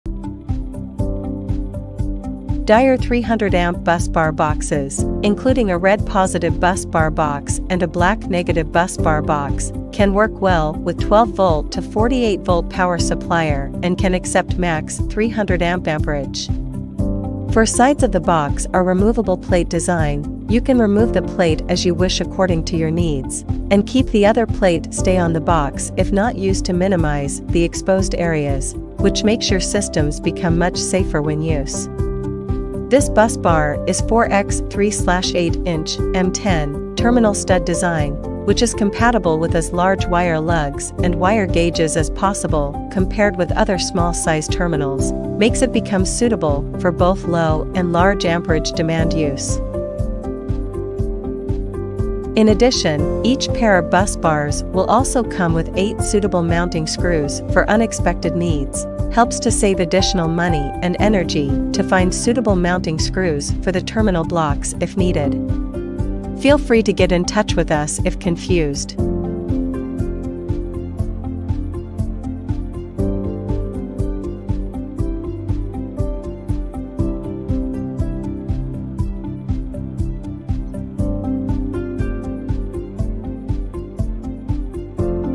12-48VDC 4 × M10 Studs Heavy Duty Bus Bar Box - How to Wire a Busbar
💦Daier BB300-T4M10-C-BR-BOX Heavy Duty Bus Bar Box

📌Specification:
Base & Cover Material: Glass Fiber reinforced Nylon
Bus Material: Brass and Nickel plated
Working Voltage: DC 12V - 48V
Max Operating Voltage: DC 48V
Max Continuous Currents: 300A
Terminal Stud Size: 4x M10
Dimensions (L x W x H): 138 x 69 x 45mm (5.43 x 2.72 x 1.77 inch)
Weight: 12.8 ounce
Application: It is widely used in power supply systems of automobiles and boats.

🔹Sturdy & Durable - The battery distribution block is made of brass and nickel-plated for good electrical conductivity and rugged rust resistance. The base is made of nylon-fiberglass reinforcement for your protection, ensuring great mechanical strength and durability.

🔹Safety Protection - The power distribution block has four removable side panels and an insulated back cover. It is designed to resist rain and dust, air, and seawater.

🔹Unique Design - The battery terminal base has a non-slip sponge for added stability. The blank writing space on the top allows easy labeling of each branch.

🔹Easy Installation - The terminal block is mounted with screws on the surface and four mounting holes make the block easier to install in any direction. The removable window allows for unobstructed wiring.

Do you want to make your wires look less cluttered? Want a more convenient wiring configuration? Use our heavy duty busbar boxes to keep you out of these hassles.